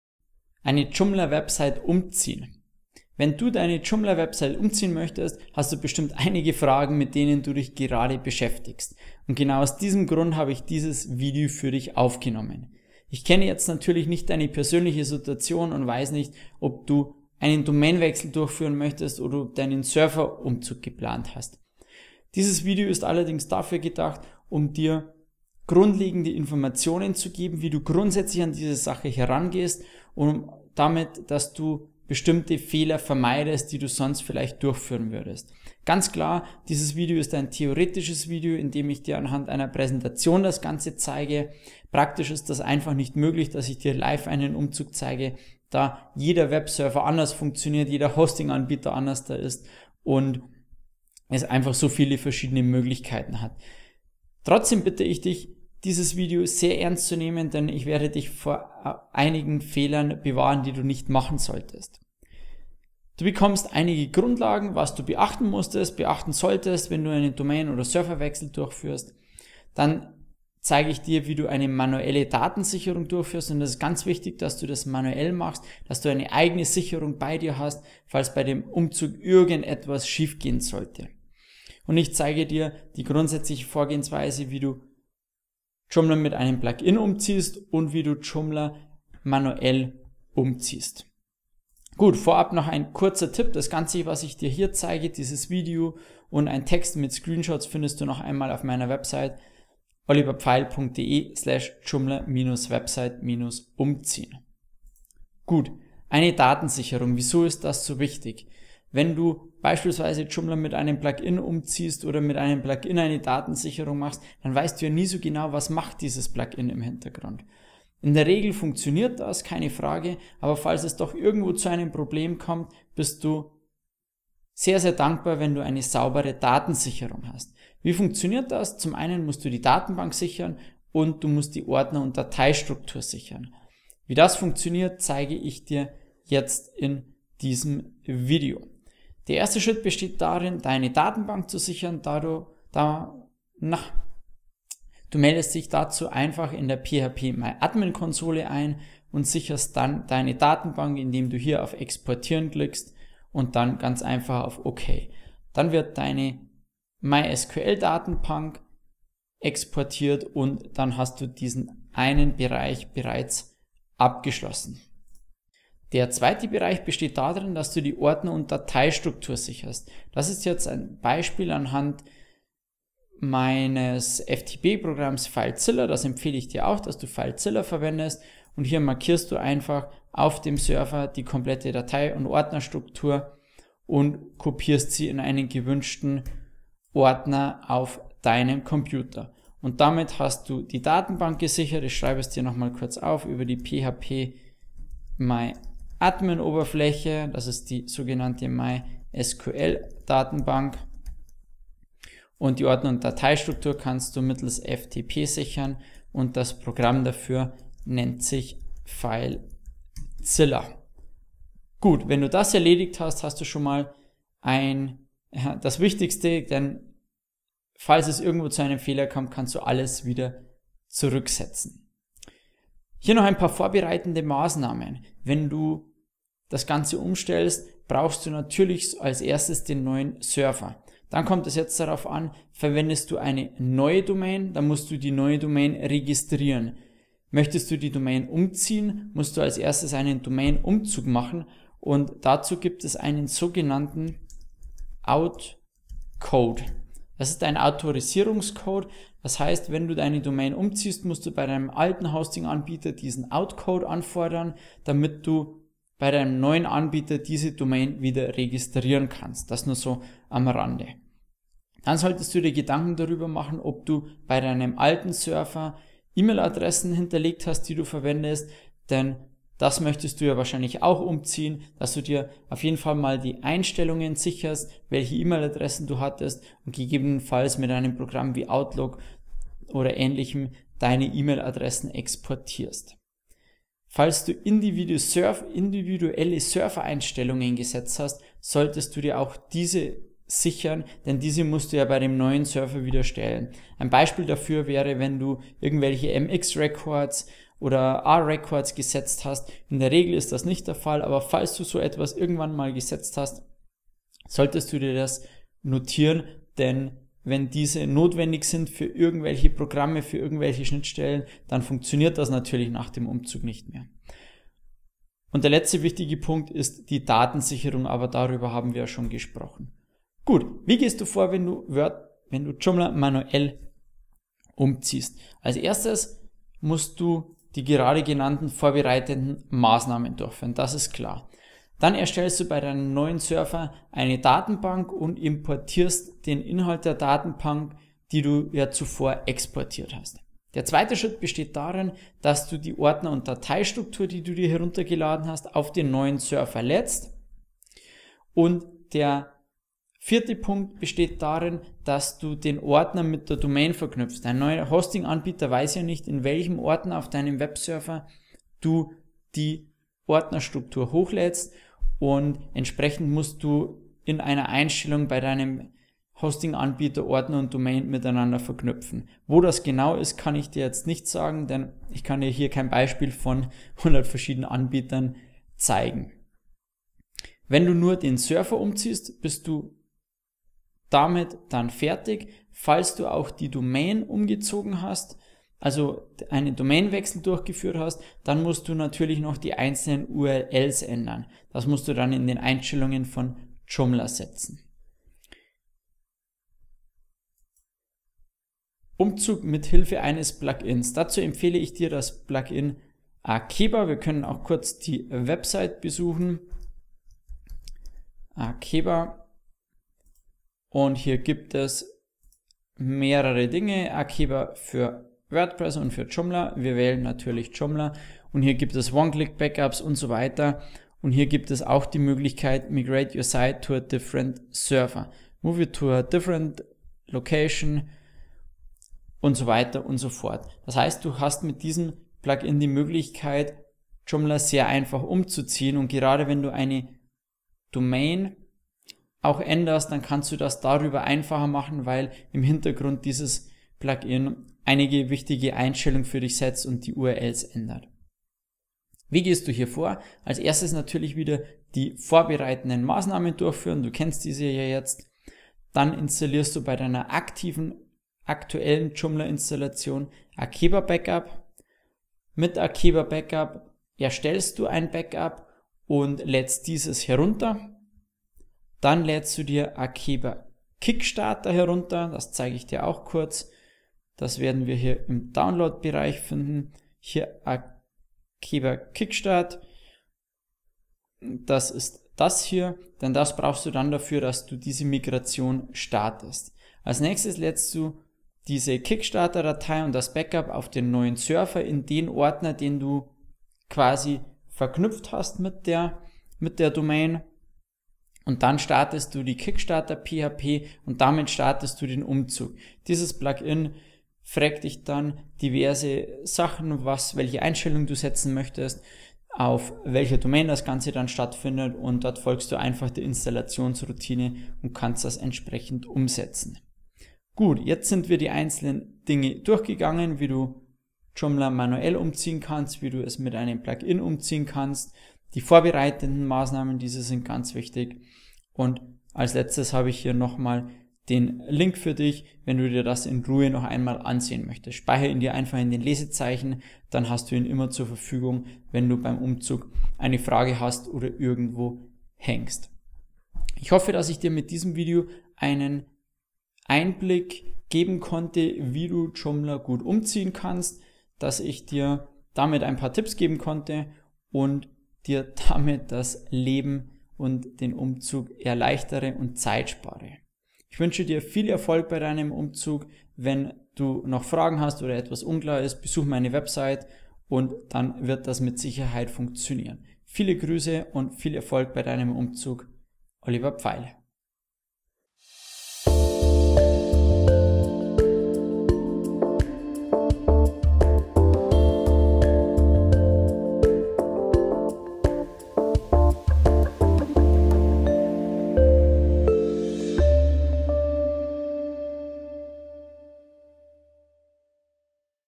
Joomla Website umziehen: Umzug Schritt für Schritt erklärt (für Joomla 3.x) 🇩🇪
✅ In diesem Video zeige ich dir den einfachsten Weg, wie du eine Joomla Website umziehen kannst. Netter Nebeneffekt? Du machst automatisch ein Backup deiner Joomla Website.

▬▬▬▬▬▬▬▬ Joomla ▬▬▬▬▬▬▬▬

▬▬▬▬▬▬▬▬ Inhaltsverzeichnis ▬▬▬▬▬▬▬▬

00:00 Joomla Website umziehen
01:31 Tipp
01:45 Datensicherung durchführen
03:40 Vorbereitende Maßnahmen
05:19 Umzug manuell durchführen
06:37 Umzug mithilfe eines Plugins (Akeeba)

Du möchtest deine Joomla Website umziehen? Den Joomla Umzug kannst du entweder manuell oder mithilfe eines Plugins durchführen.

Wichtig ist, dass du vor dem Umzug deiner Joomla Website eine Datensicherung, also ein Backup durchführst.

Folgende Schritte musst du durchführen, wenn du deine Joomla Website umziehen möchtest:

1. neues Hostingpaket (Server) bestellen
2. Domain umziehen oder neue Domain beantragen
3. Joomla Backup durchführen (Datenbank & Ordnerstruktur sichern)
4. auf dem neuen Server eine Datenbank erstellen und die Datenbanksicherung hochladen
5. Ordnerstruktur auf den neuen Server laden.
6. Verknüpfung zwischen Domain und Ordnerstruktur herstellen.

▬▬▬▬▬▬▬ Mehr Informationen ▬▬▬▬▬▬▬

▬▬▬▬▬▬▬▬ Hashtags ▬▬▬▬▬▬▬▬

▬▬▬▬▬▬▬▬ Impressum ▬▬▬▬▬▬▬▬